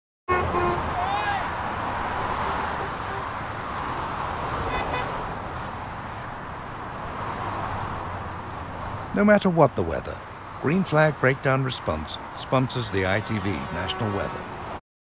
Green flag ' fancy dress'
This is one of five commercials for Green Flag, one of the UK’s leading breakdown companies. We worked with CHI & Partners and production company Minds Eye Media, with footage shot on a light day in May, and completed using a mixture of Smoke & Grade (Da Vinci Resolve) to create various weather conditions. The collection of commercials can all be seen on ITV over the course of the next three months.

Agency - CHI & Partners
Brand: Green Flag
Production Company - Minds Eye Media
Happy Finish Online - Will Yarrow
Happy Finish Colourists - Ned Al-Astrabadi & Kenny Gibb